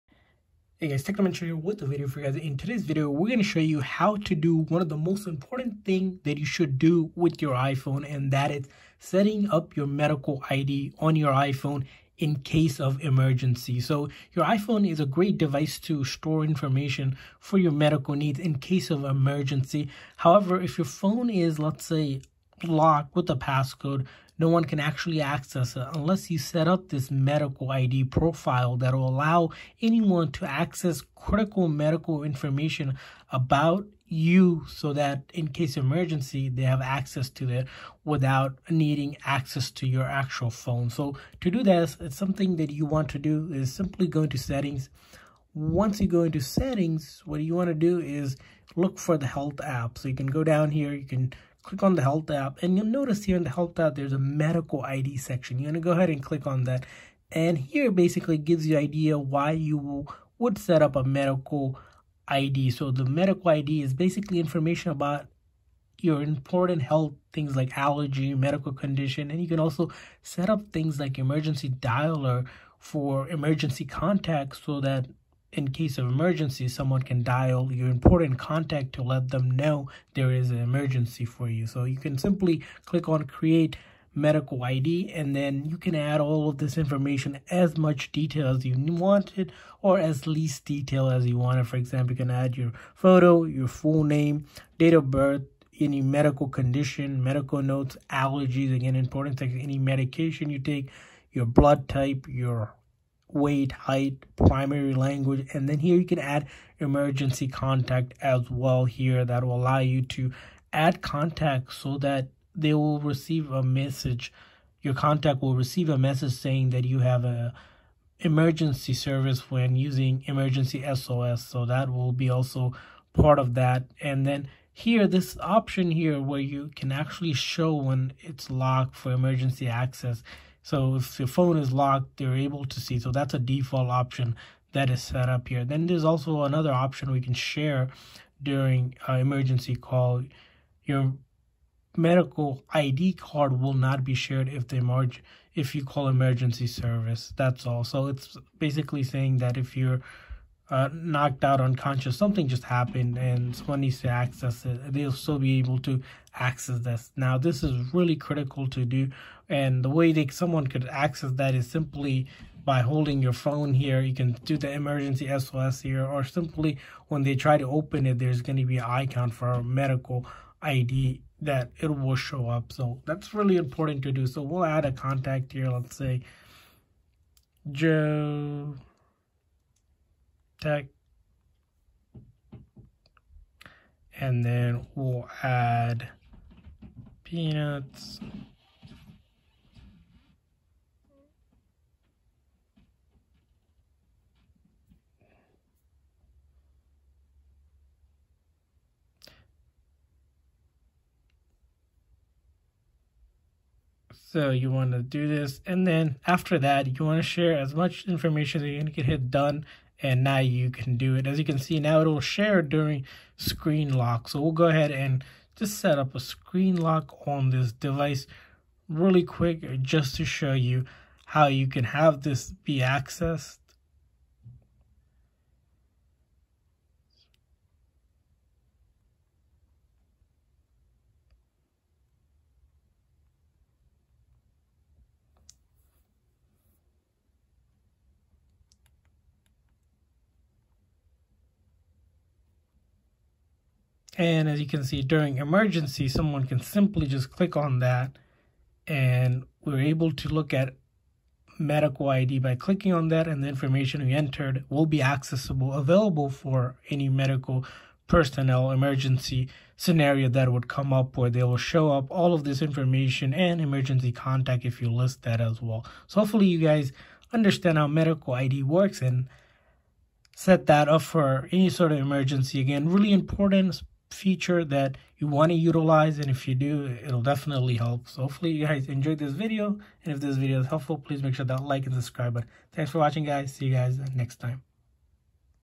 How to Set Up Emergency Medical ID on iPhone
In this tutorial, we show you how to set up Medical ID on your iPhone for emergency.